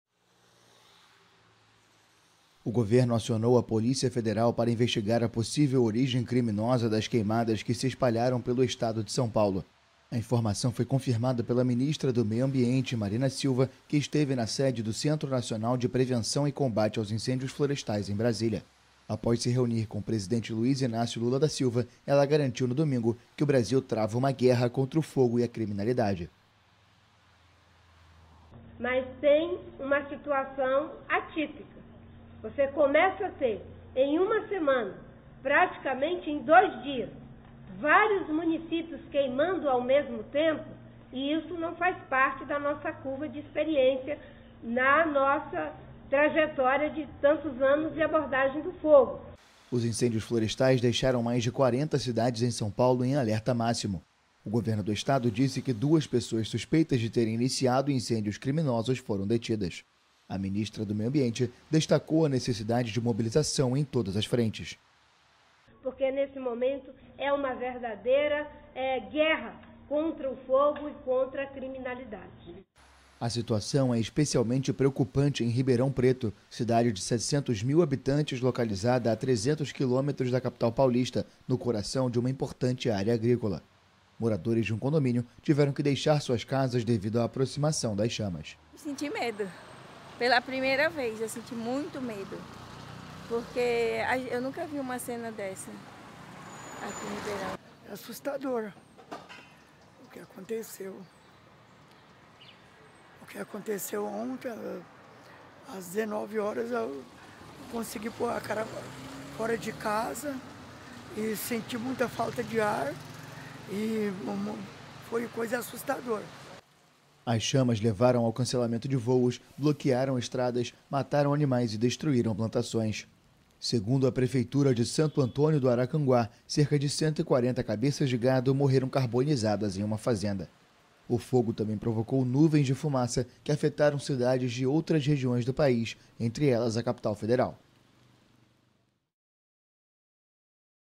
GOVERNO FEDERAL SUSPEITA DE INCÊNDIOS CRIMINOSOS NO PAÍS
A ministra do Meio Ambiente, Marina Silva, garantiu que o Brasil trava uma ‘guerra contra o fogo e a criminalidade, em meio aos incêndios florestais que atingem o estado de São Paulo e que deixaram mais de 40 cidades em alerta máximo.

Créditos: Canal Gov/AFP